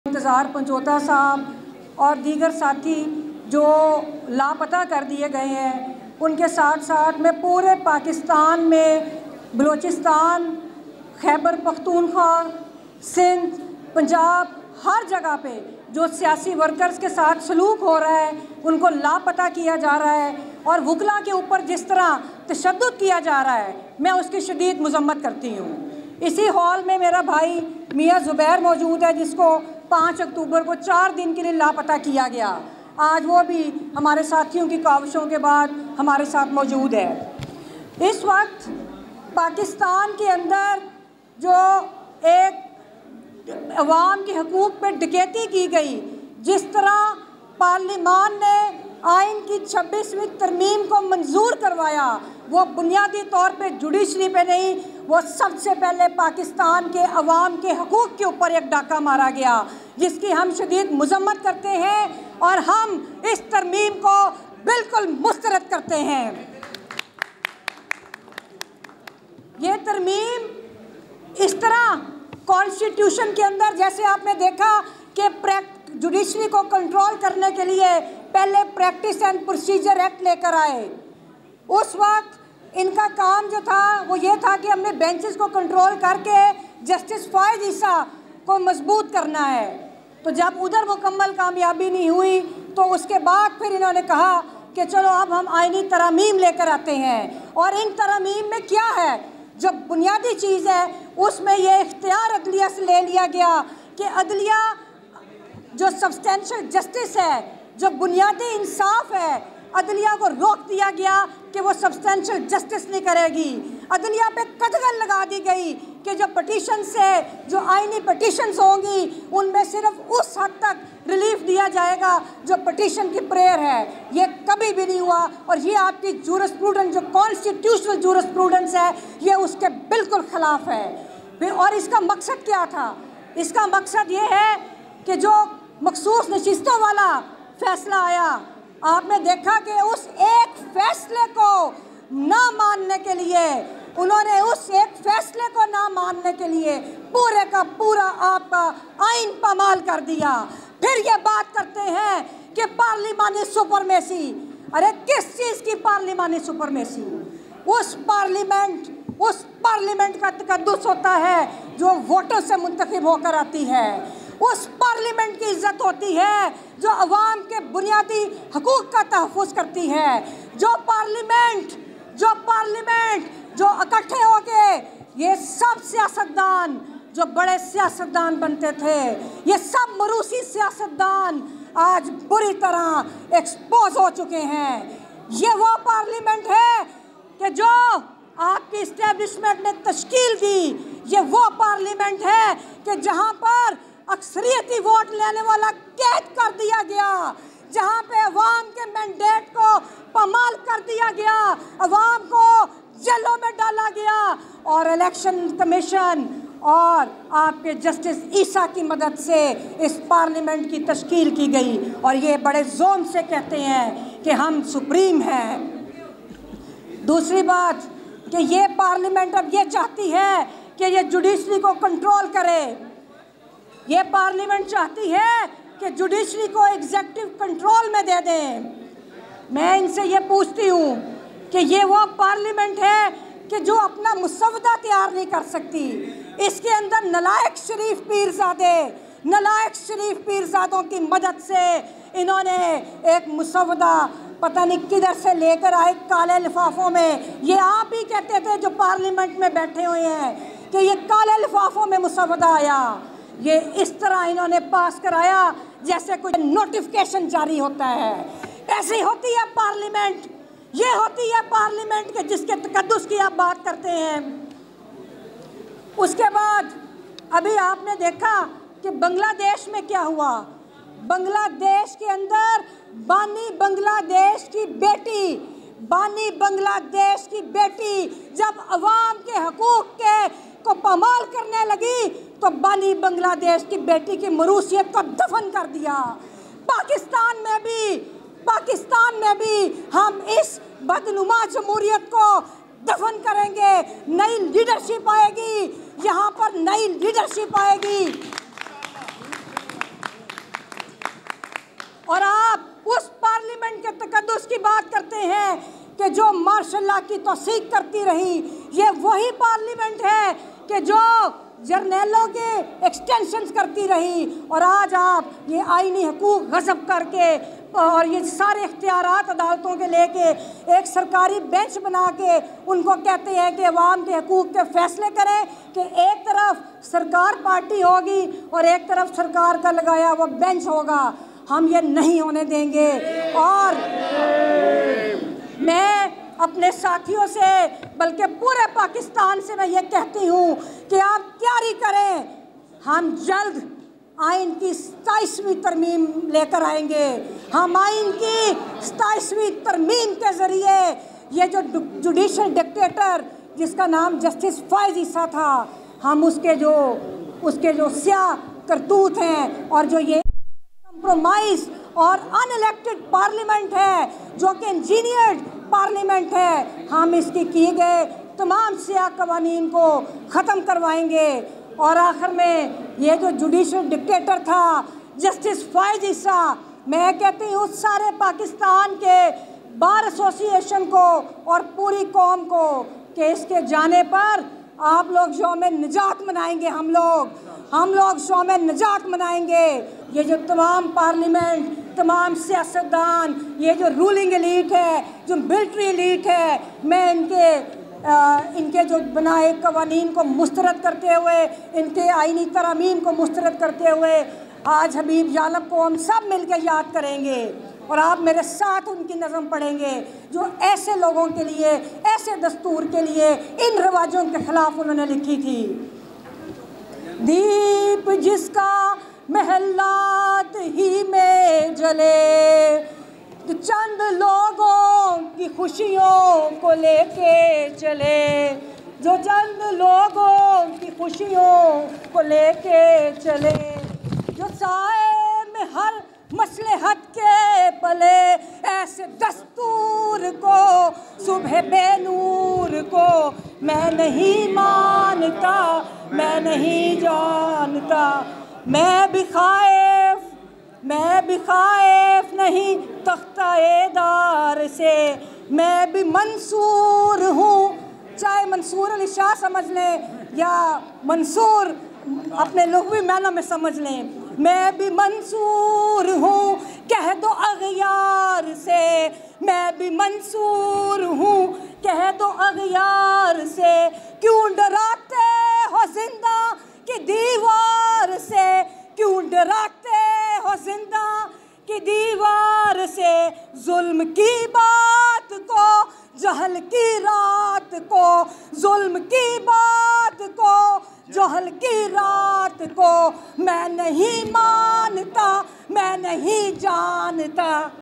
Lawyer Rabbiya Bajwa Blasting Speech | Constitutional Amendments | Qazi Faez Isa | CurrentNN
| News Updates